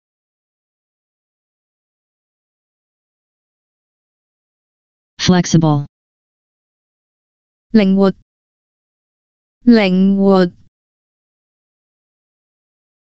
"FLEXIBLE" in Cantonese (靈活) - Flashcard
"FLEXIBLE" in Cantonese (靈活) - Flashcard. Additional information about the word: nimble,agile,flexible,elastic.